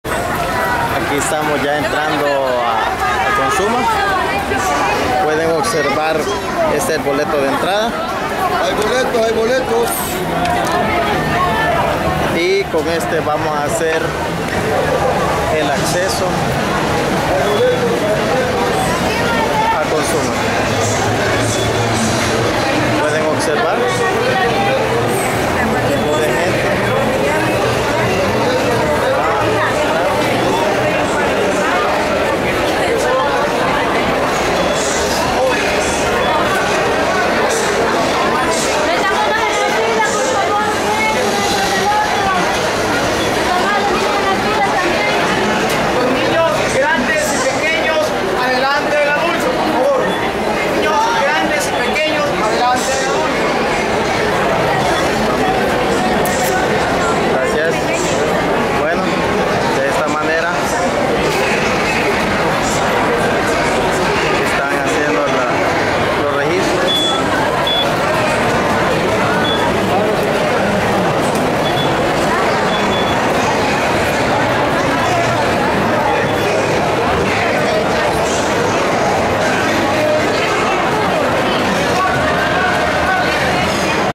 [ YOUTUBERO ] Consuma CIFCO SAN SALVADOR 2011 | 2º parte
[CONTACTANOS] ... Este Reportaje esta dedicado a Tod@s los Salvadoreñ@s que extrañana las tradiciones de nuestra Tierra El Salvador , especialmente para mis Paisanos Quezaltecos que residen fuera de nuestro país y que a pesar de no poder tener acceso a volver facilmente les llevo de todo corazon este pequeño reportaje